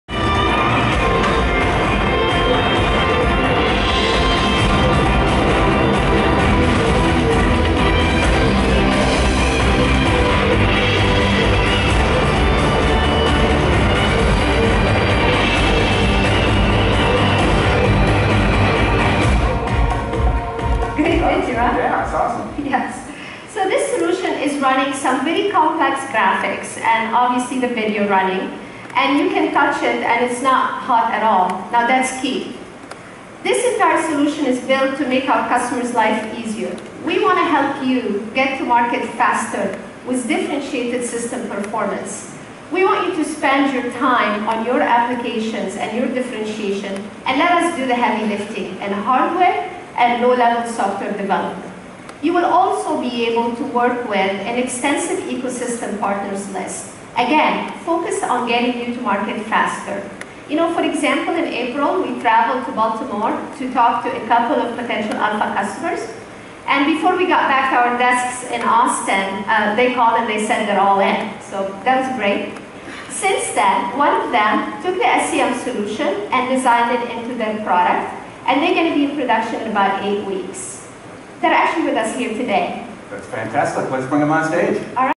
FTF 2015 1st Day Keynote #2
Freescale Technology Forum 2015 America Day One Keynote : All New i.MX Processor Launch (Record : Acrofan)